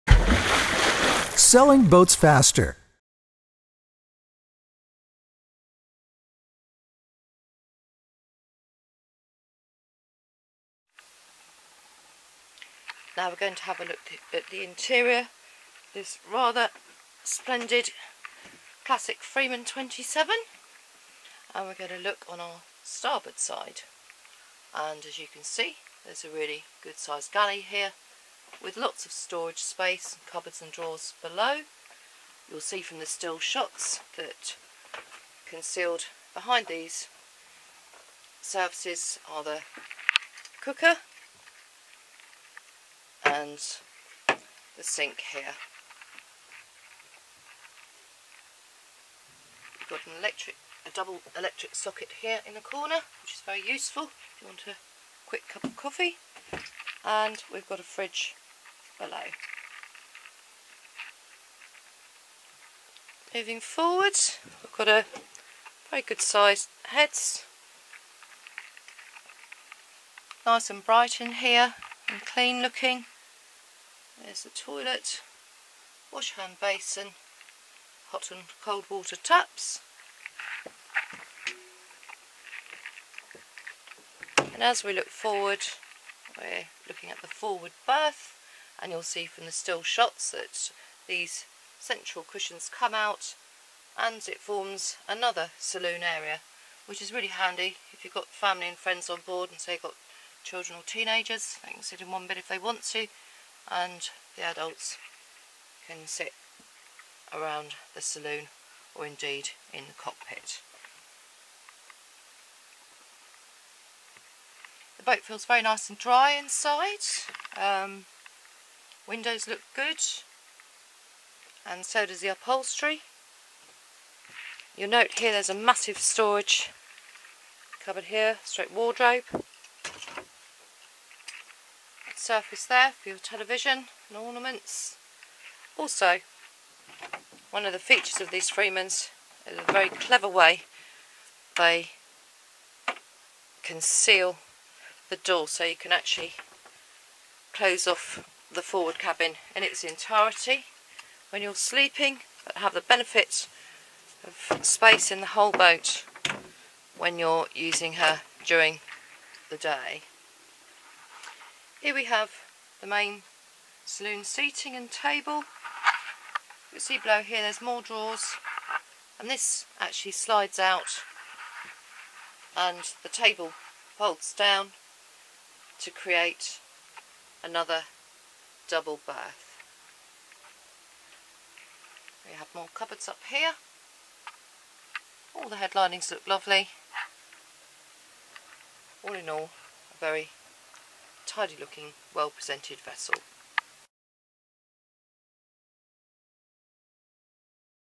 Freeman 27 - Boatshed.com - Boat Ref#166033
Freeman 27 for sale with Boatshed Medway Photos and video taken by Boatshed Medway